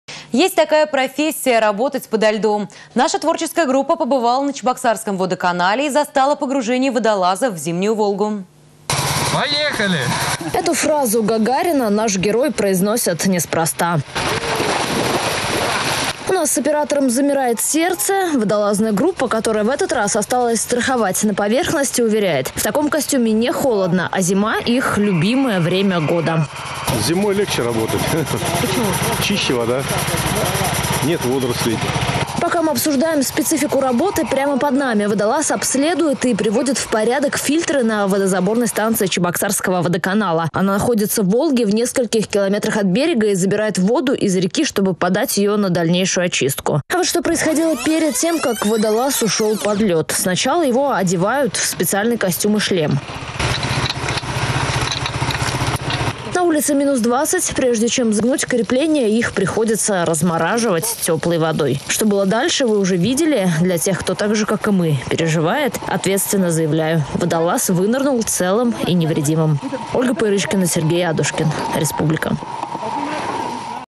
Есть такая профессия — работать подо льдом
Наша творческая группа побывала на Чебоксарском Водоканале и застала погружение водолазов в зимнюю Волгу.